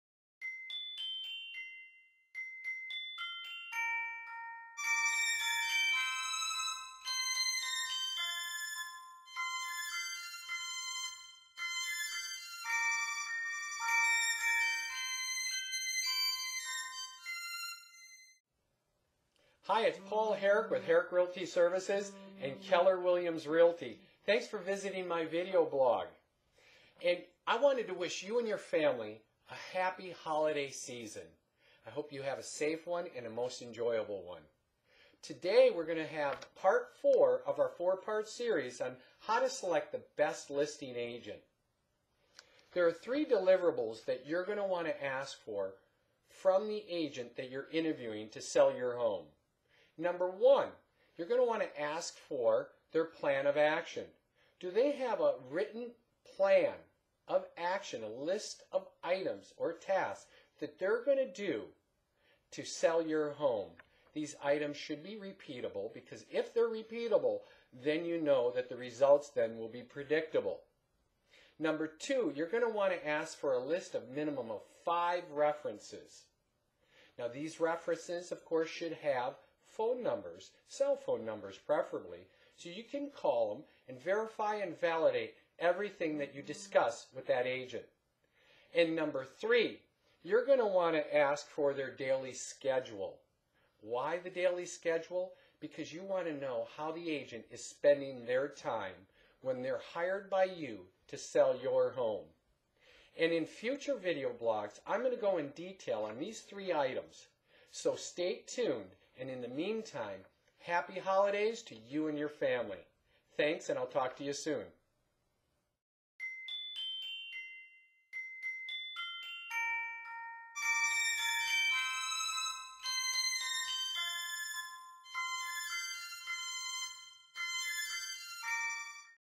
How to Select the Best Listing Agent - Part 4
Happy Holidays to you and yours!! I've concluded my "Best Listing Agent" series with some tips on what deliverables you should expect when choosing who you work with.

Paul Herrick
Herrick Realty Services